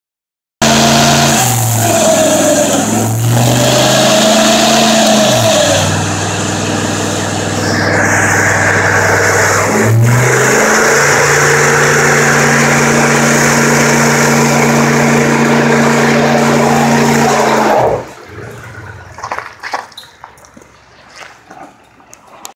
6.5 suburban burnout getting New tires
New tires came in tried to make old ones go out with a bang no luck